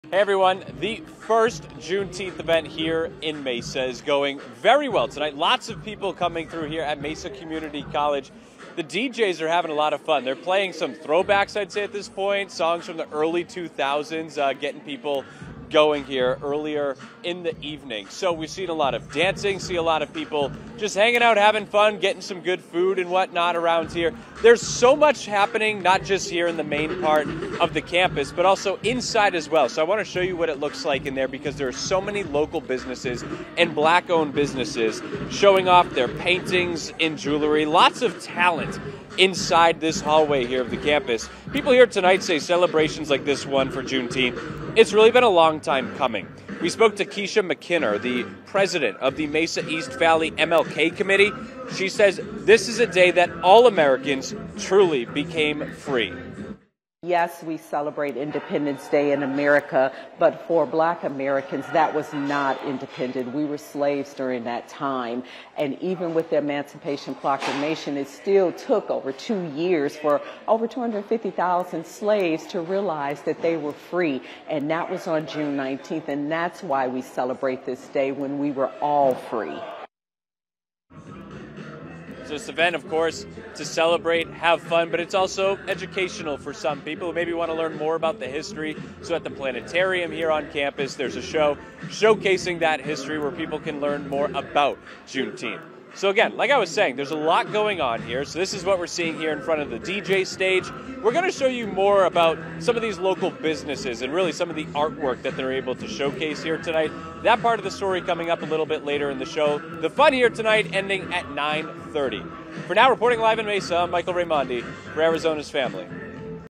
Mesa holds first ever Juneteenth celebration
The city of Mesa celebrated Juneteenth with a special festival on Wednesday at Mesa Community College.